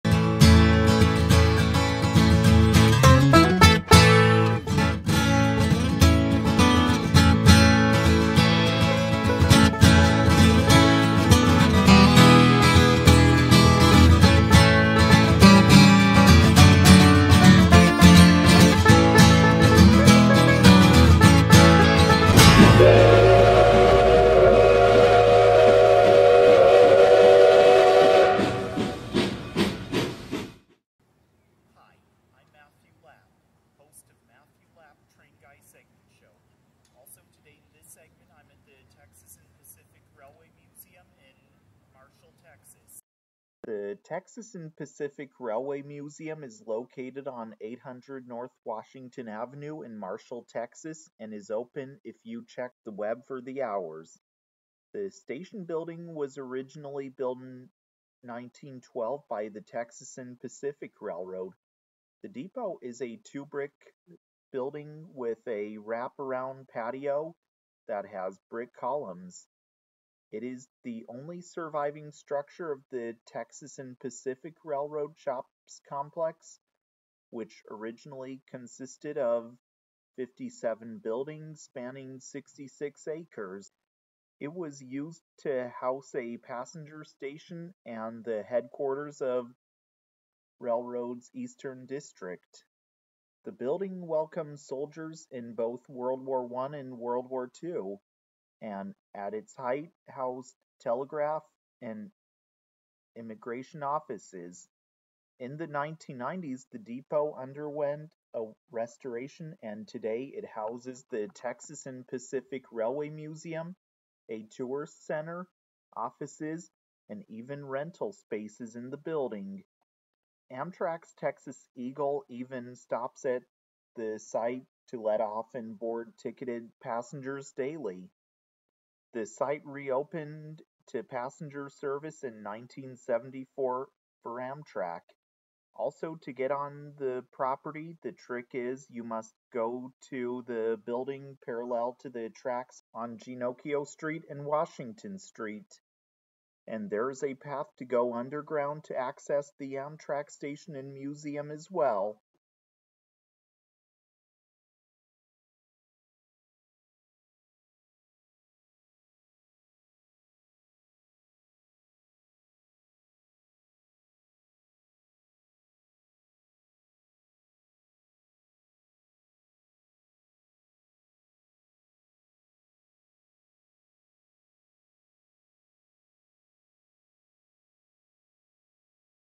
_Texas & Pacific Railway Museum – Marshall, TX_ Episode 343 (Beginning/T&P Depot)
Texas & Pacific Railway Museum
Historic Train Depot with Museum and Amtrak service in Marshall
_MLTG_